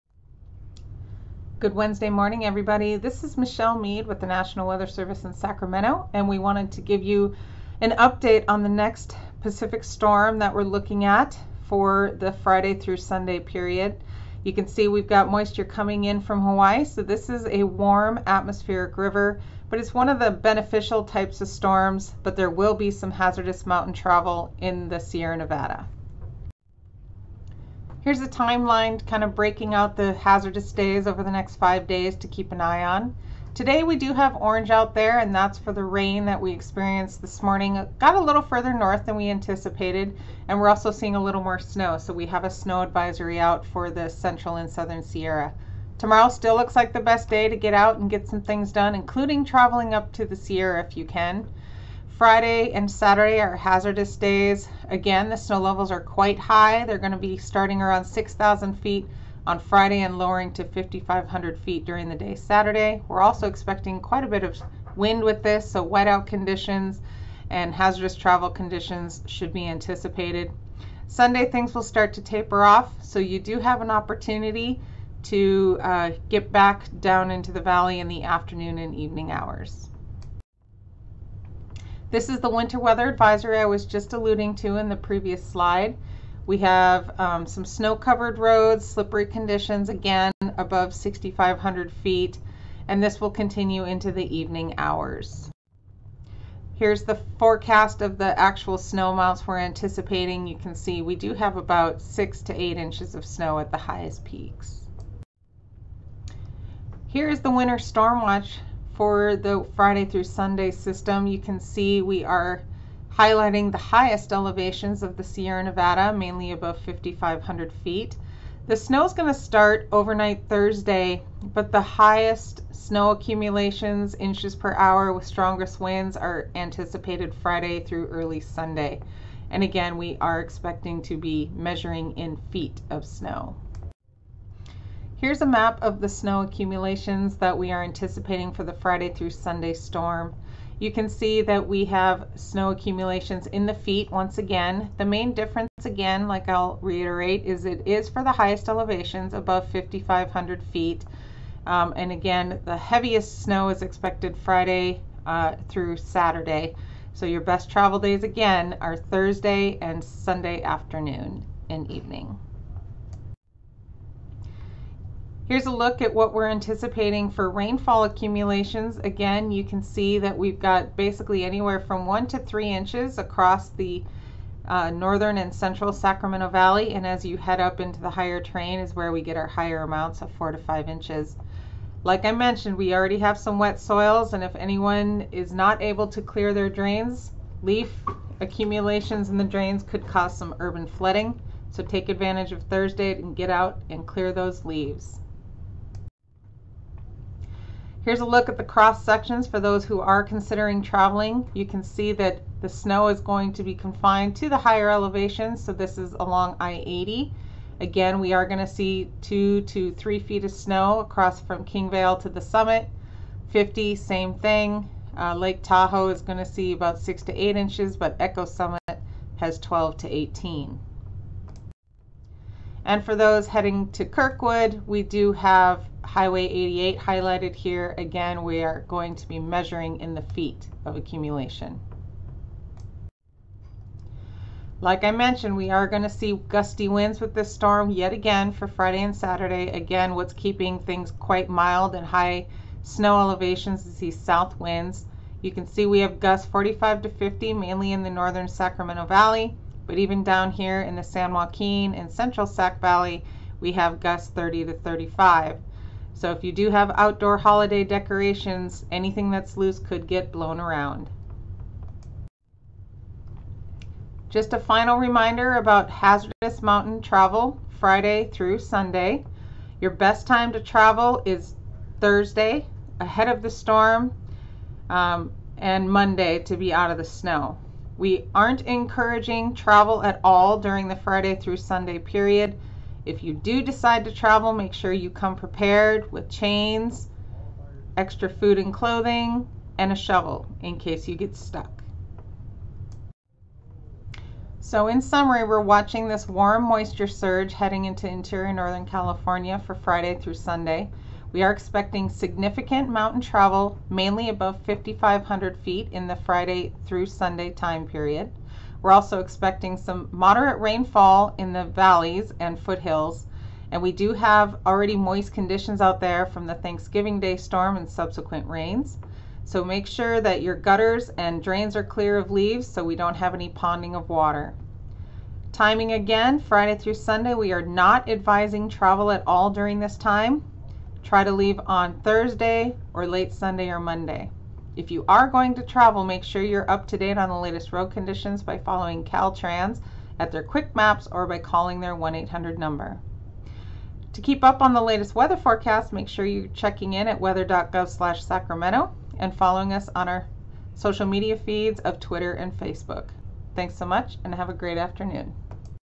Winter Storm December 6-8, 2019 Weather Briefing
Heavy snow and gusty winds will create hazardous mountain travel Friday November 6 - Sunday November 8, 2019. This atmospheric river event will create high snow levels above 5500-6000 feet as well as moderate to heavy Valley rainfall.